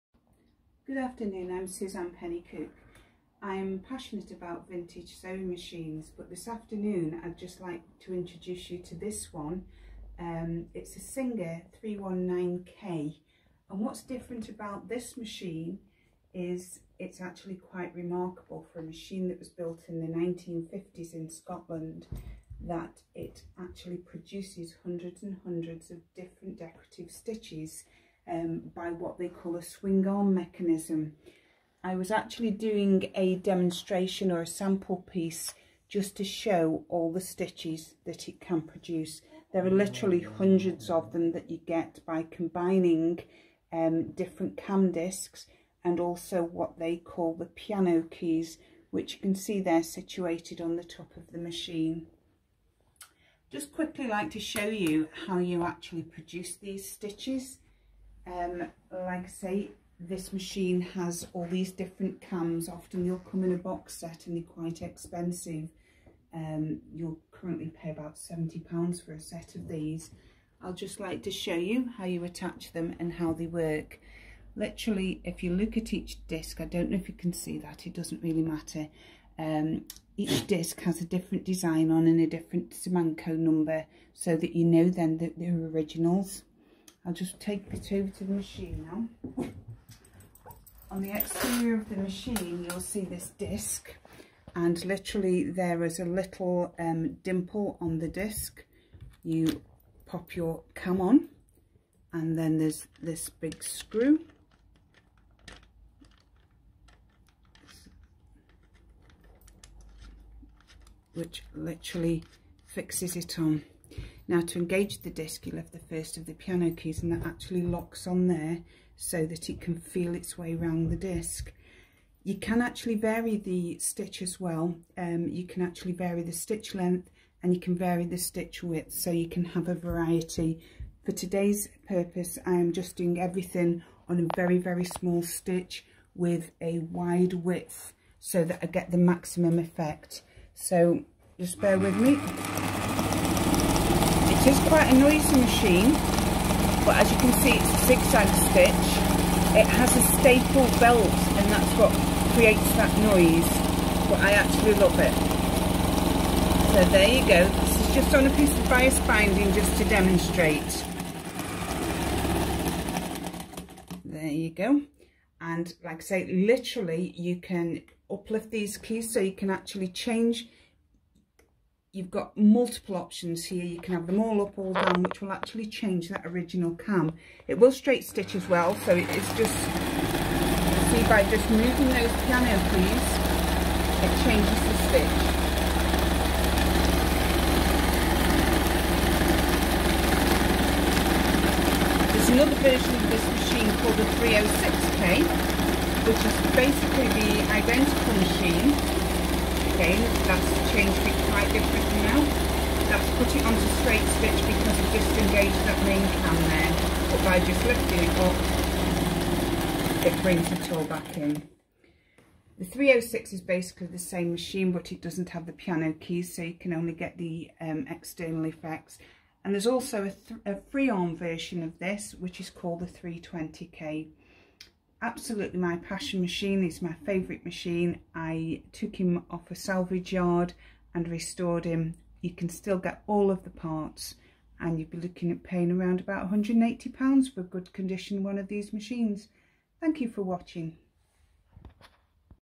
The Singer 319k for embroidery stitches
A short clip about how a swing needle vintage sewing machine uses fashion cams to create a huge range of embroidery stitches.